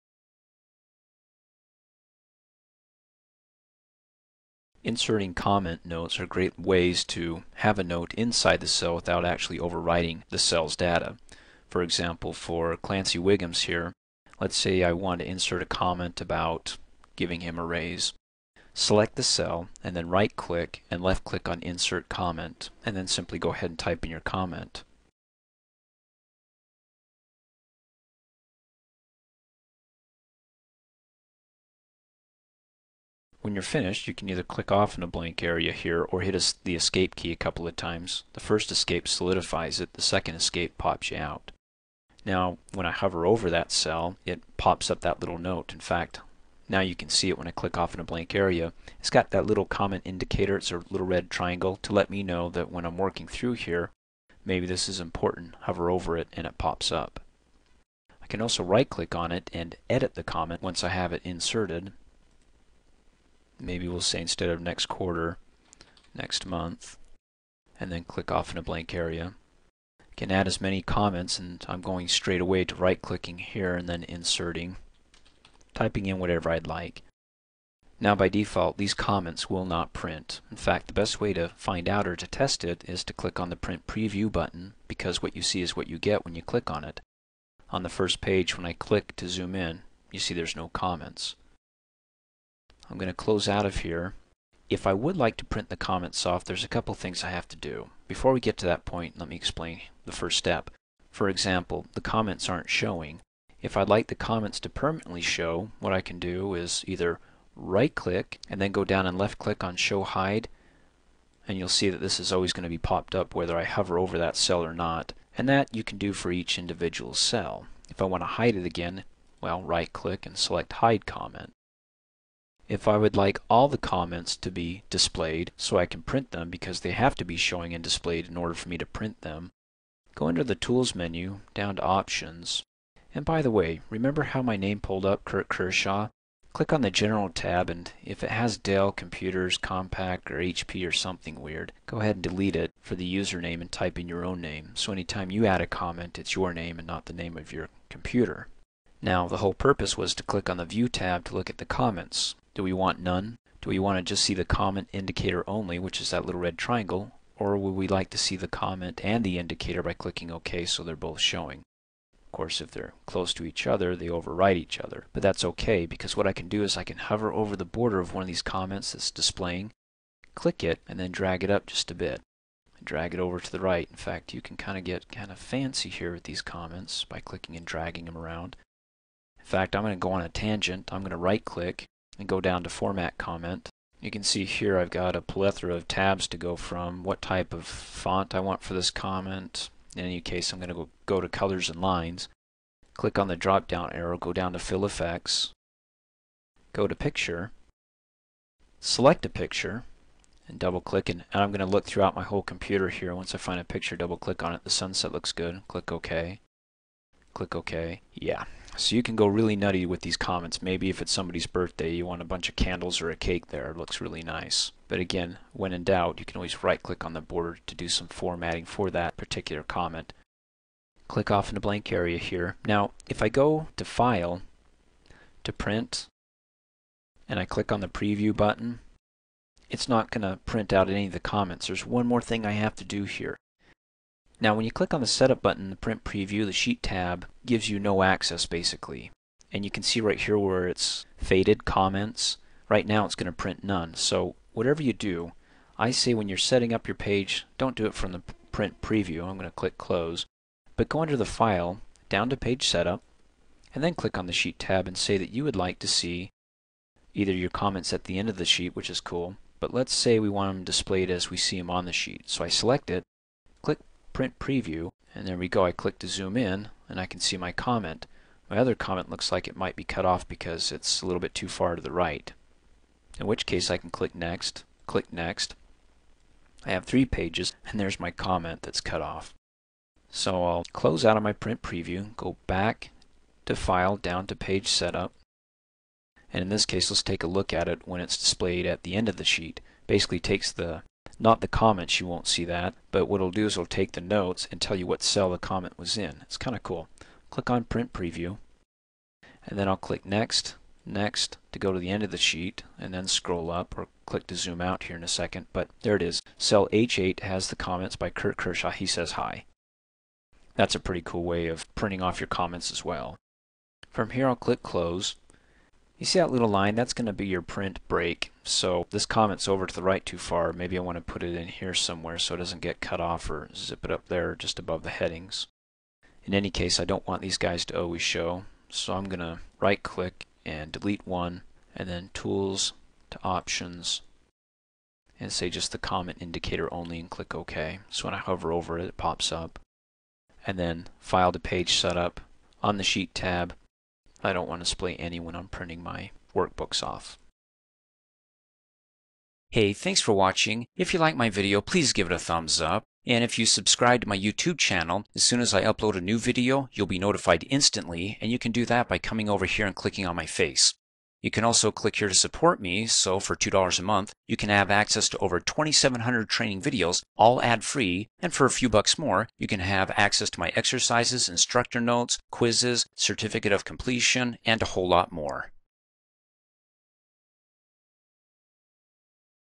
Excel 2003: How to Insert, Edit, Delete Comments
Excel 2003 tutorial on how to insert, edit and delete a comment in a cell in this Excel training video on comments!

All 80 of my Excel 2003 training videos contain everything you need to help pass the Microsoft Office Excel 2003 exam and become certified!